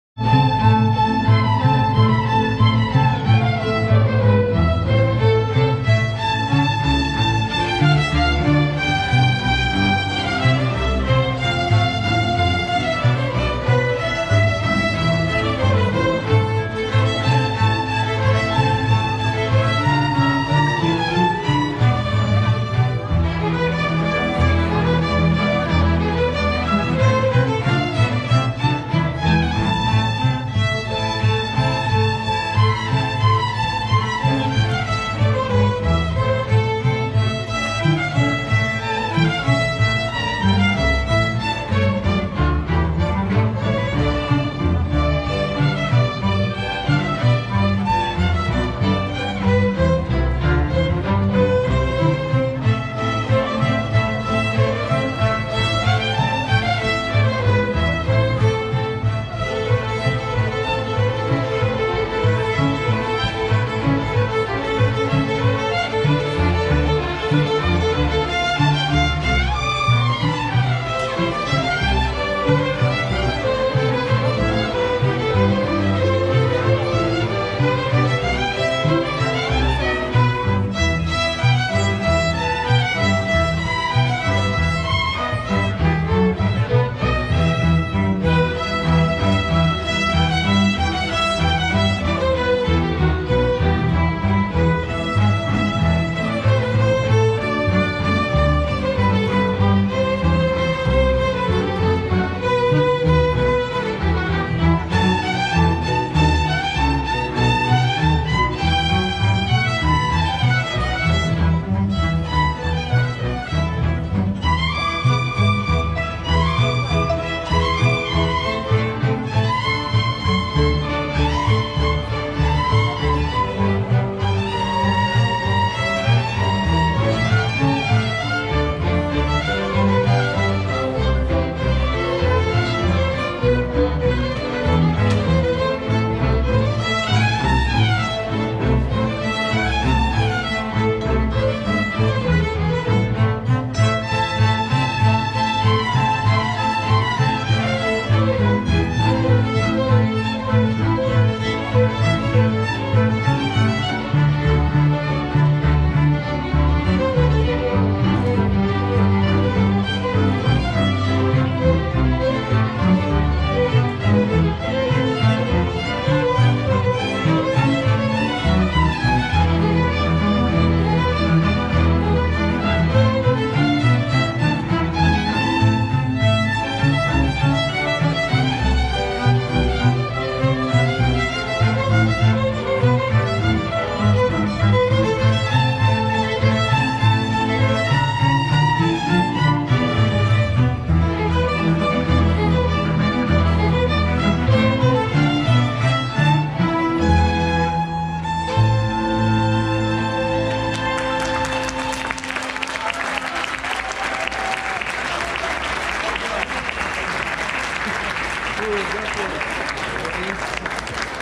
Encontro de jovens músicos canelinha e região de Santa Catarina Florianópolis, Brasil e Chile 2020
Encontro de jovens músicos canelinha e região de Santa Catarina Florianópolis, Brasil e Chile
Orquesta da cámara Viña del Mar do Chile